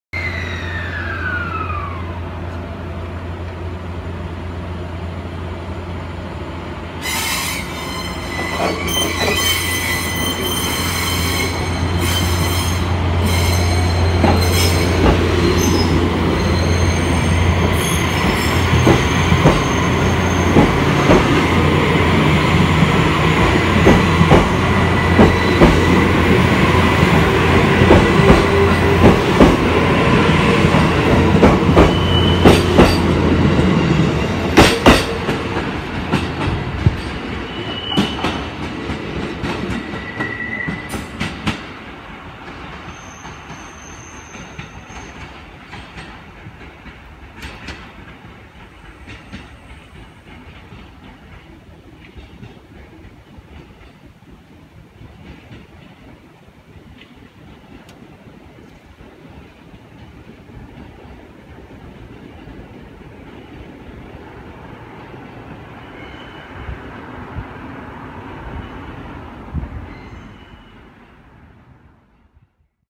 【谷川ループ号返却回送】水上駅にて E257系500番台の発車シーンを撮影
上越線:水上駅1番線ホーム 高崎寄り
撮影日: